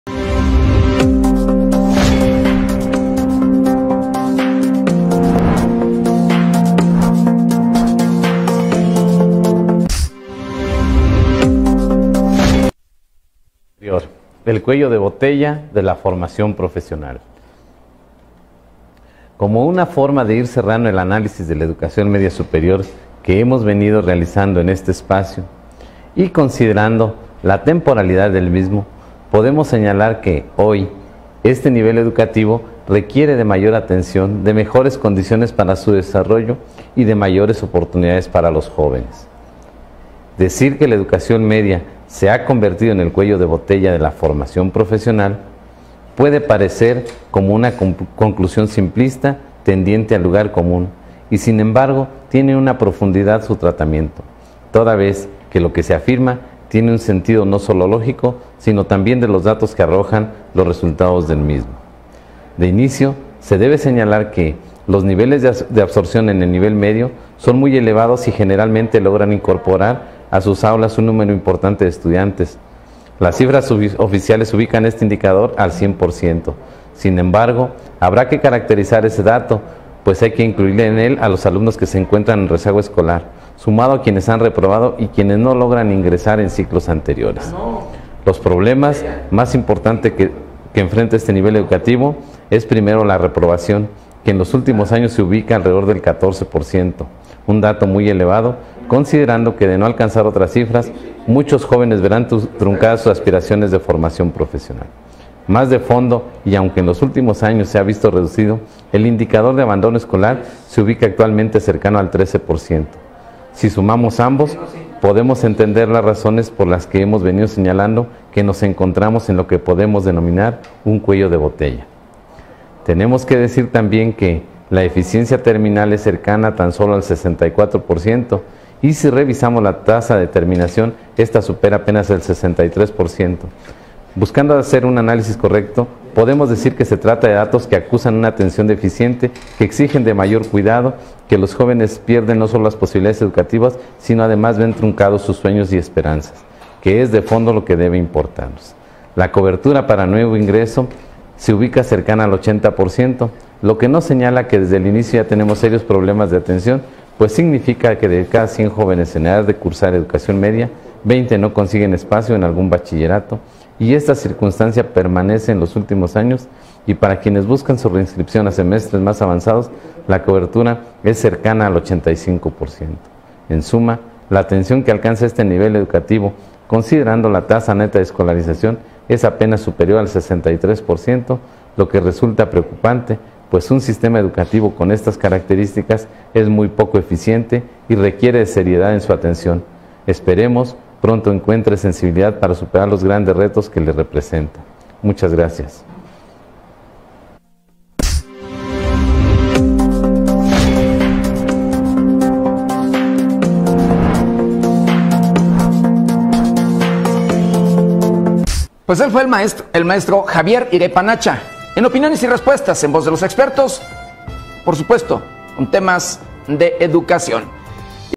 Opiniones y Respuestas En Voz de los Expertos con el Mtro Javier Irepan Hacha en Temas de Educación
Opiniones y Respuestas En Voz de los Expertos con el Mtro Javier Irepan Hacha en Temas de Educación - La educación media superior, el cuello de botella de la formación profesional